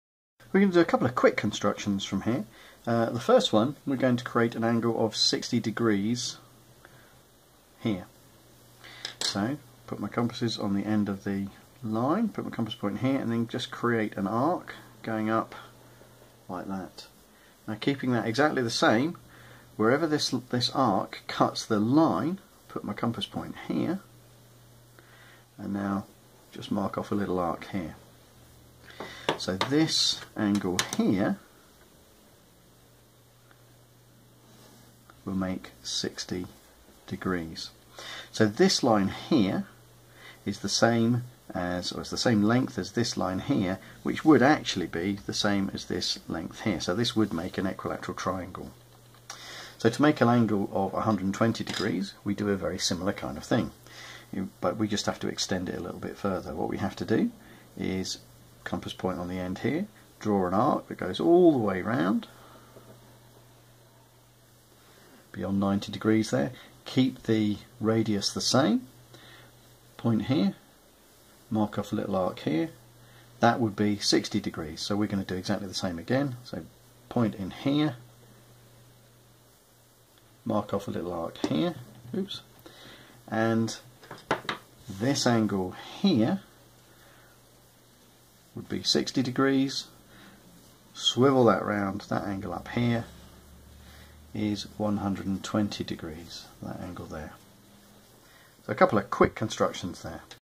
Construct 60 degree and 120 degree angles
How to use a straight edge and compasses to construct either a 60 degree or 120 degree angle. In GCSE maths constructions questions DO NOT USE A PROTRACTOR (other than to CHECK the accuracy of your answers) - remember, the word "CONSTRUCT" is pronounced "NO PROTRACTOR"